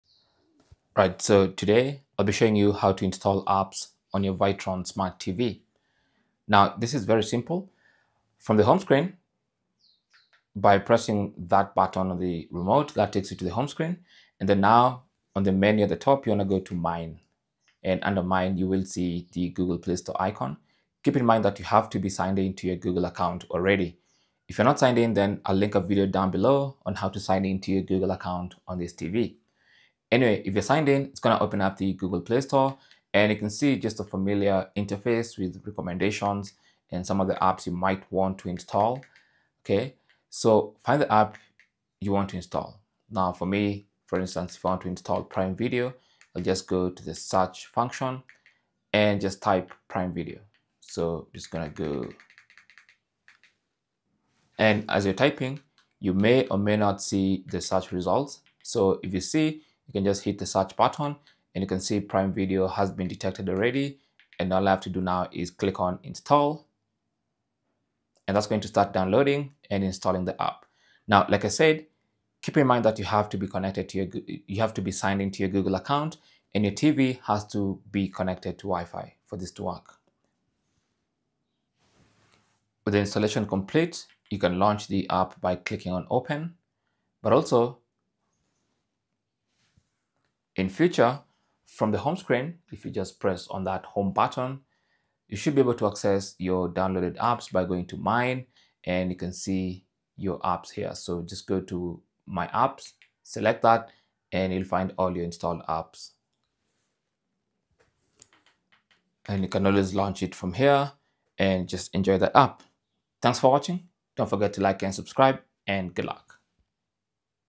How To Install Apps From Play Store On Vitron Smart TV
This guide walks you through the complete process of installing apps from the Google Play Store on a Vitron Smart TV. By the end, you will be able to set up your TV confidently and customize it with the apps you need for streaming, gaming, and productivity.

Cell Phone Tripod Adapter